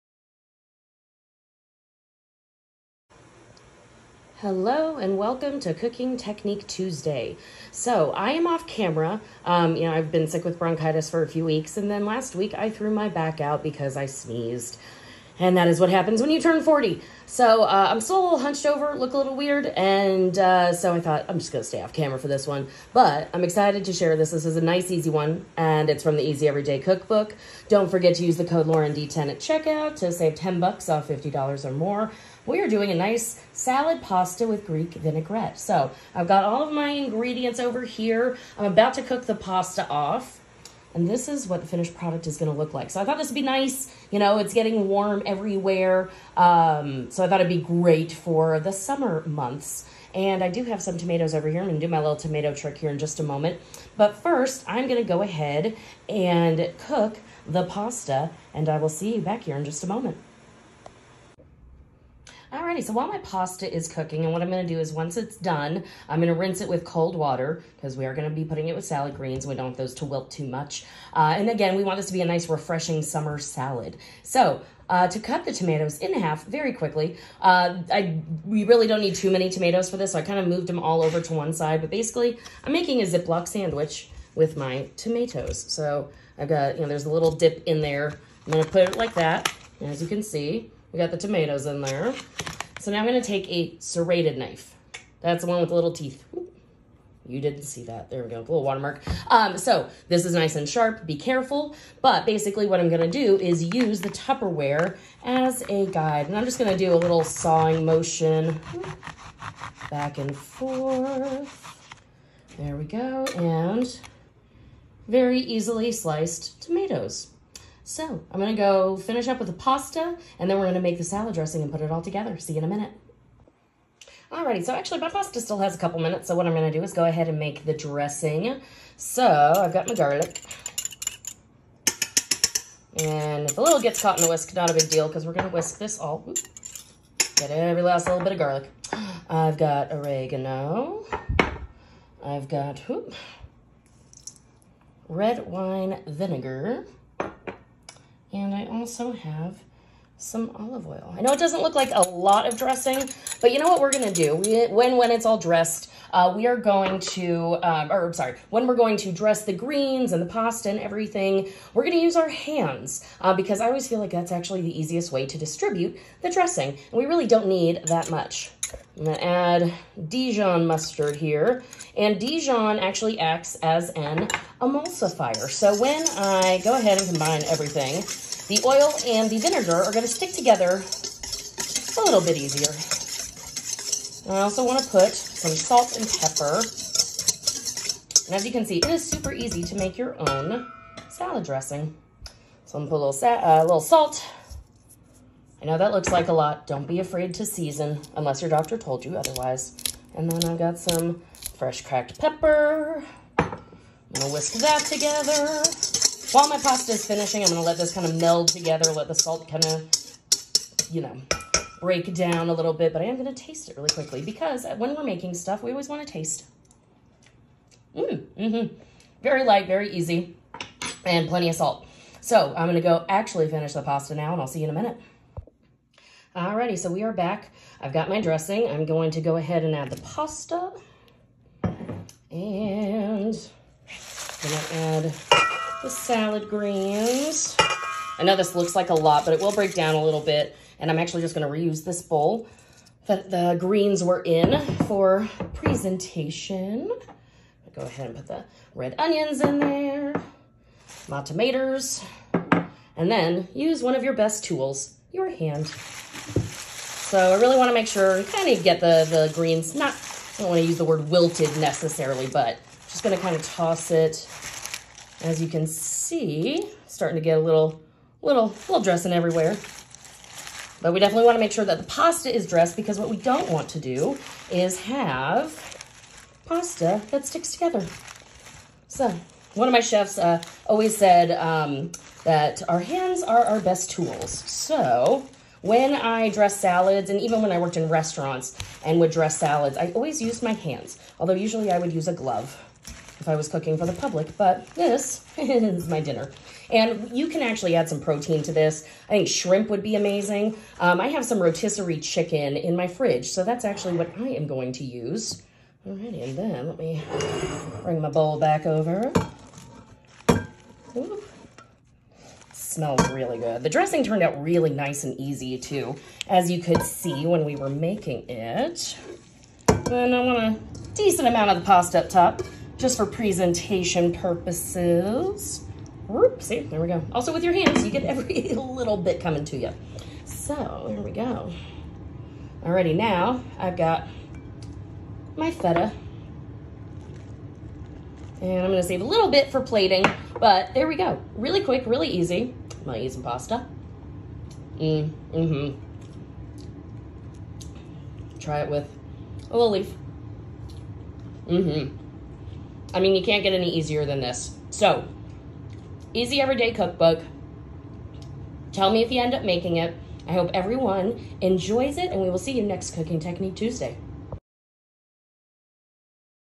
WW Salad Pasta with Greek Vinaigrette
This tossed salad–pasta salad marriage turns just 4 ounces of pasta (lentil pasta for extra protein and fiber) into something hearty. Top with feta for lots of tang. This simple vinaigrette is an all-purpose dressing that’s great on grilled shrimp and chicken too. Make a double batch to keep in the fridge, and use all week long.